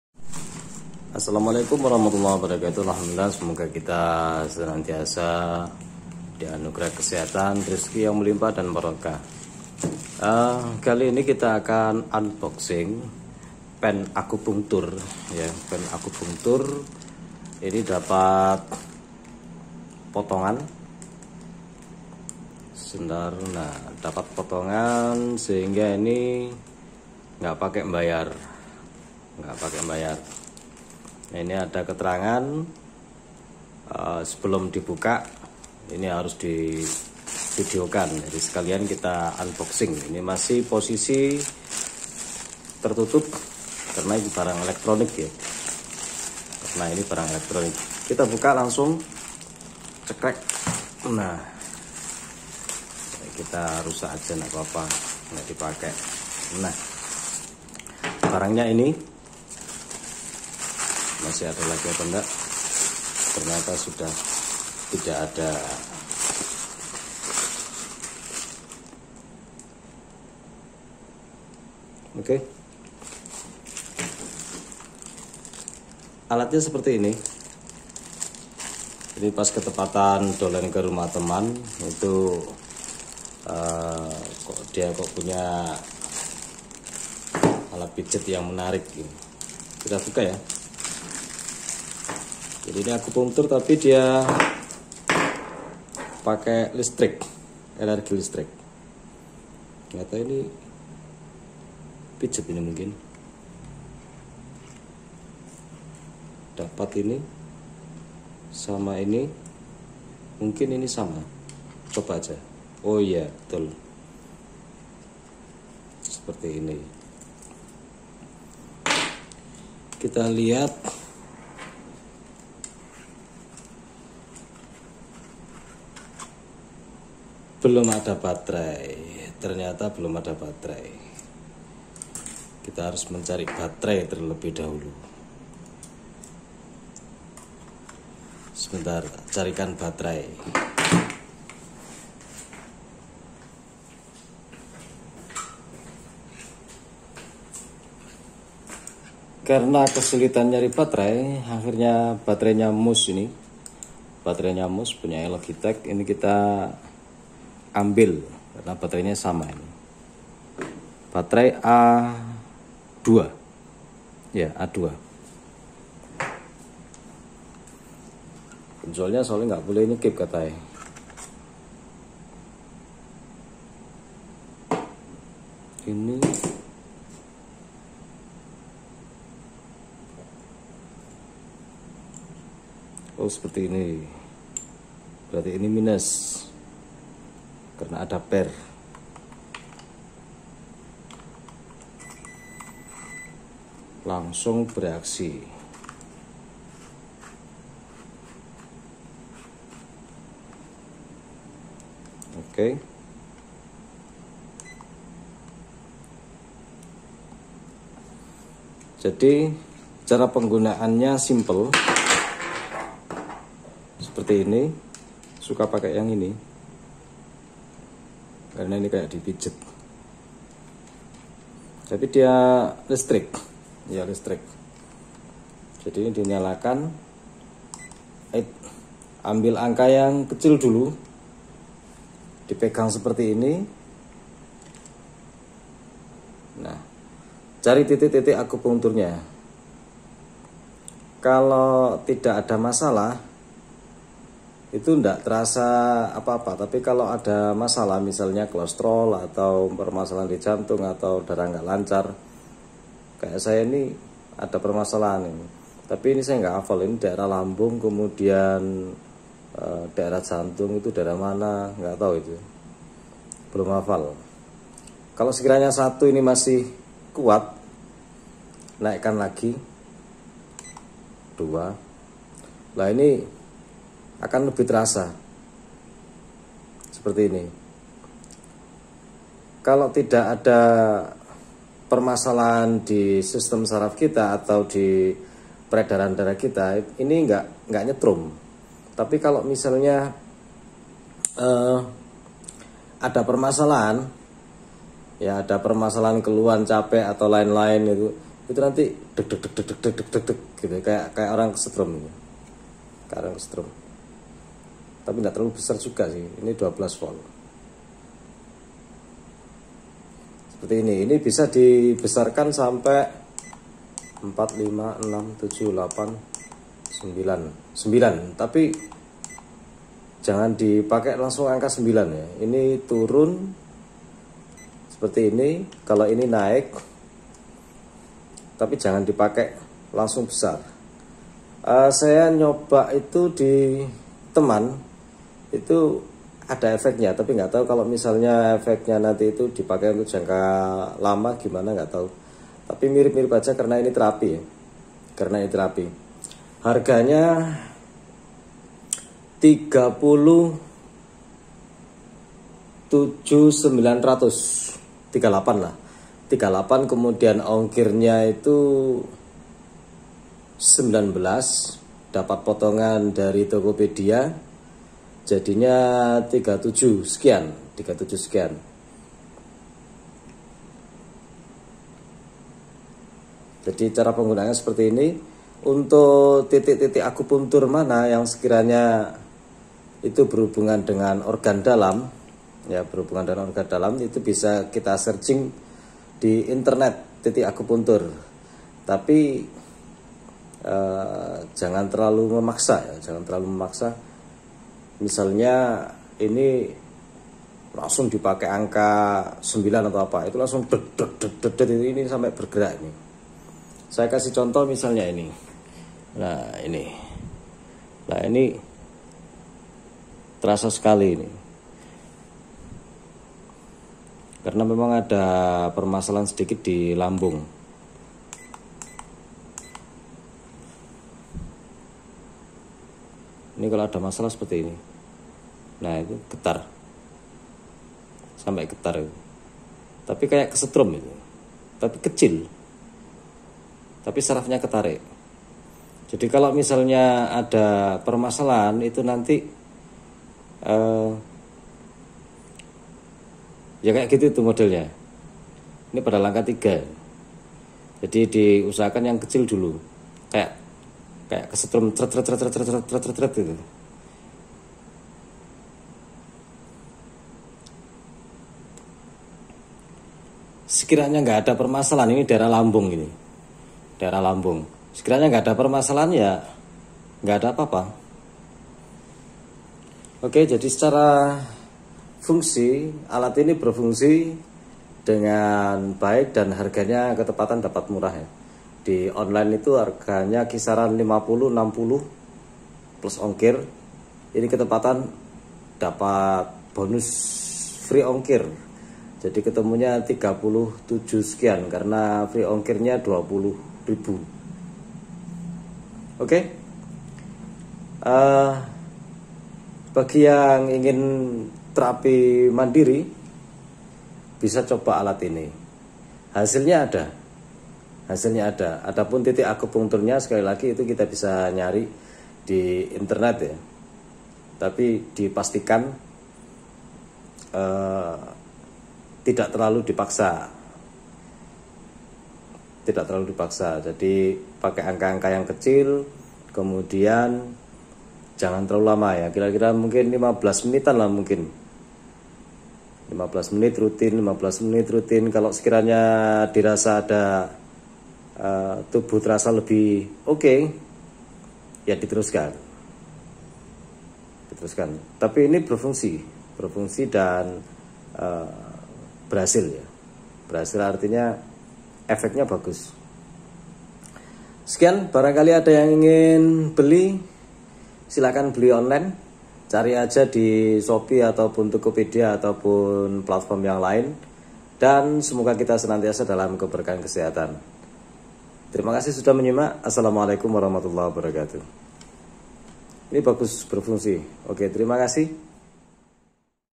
Pen akupunktur Cuman Rp. 37.299 | Normal sampai Rp. 60.000
perkiraan listrik kejut antara 12 volt, dengan catu daya 1.5 volt batrai A2.
Pembelian via tokopedia.